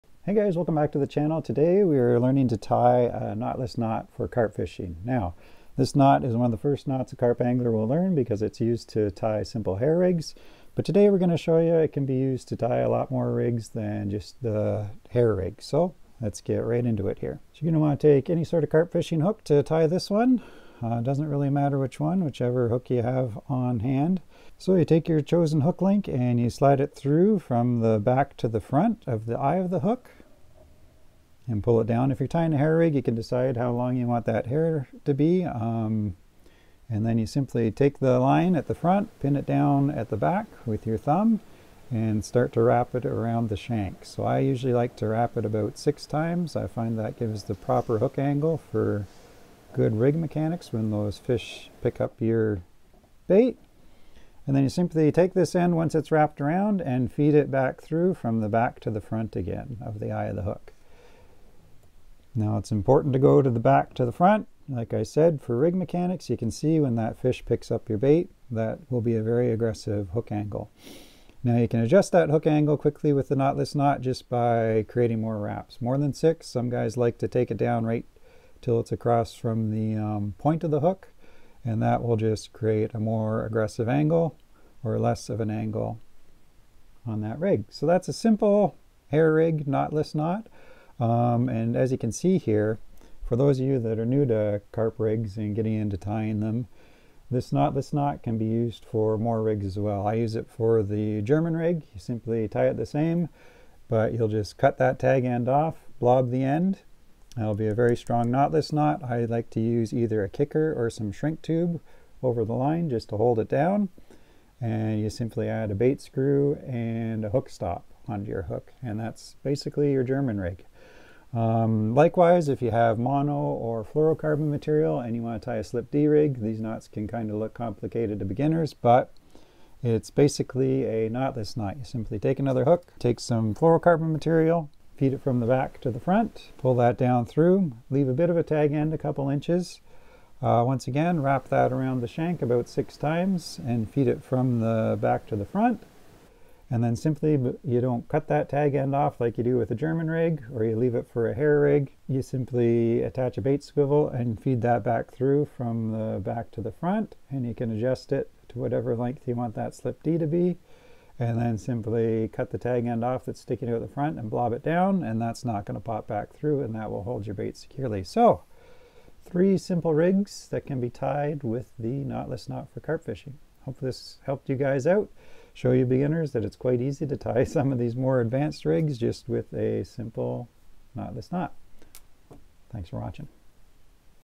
How to Tie a Knotless Knot for Carp Fishing
In this video, we quickly review How to Tie the Knotless Knot for Carp Fishing. It is a very strong and versatile knot that can be used for more than just simple hair rigs.

We demonstrate the step-by-step process for tying the knotless knot for a hair rig, german rig and even a simple slip-d rig.

For more basic information about the knotless knot and a illustrated step by step guide visit our corresponding article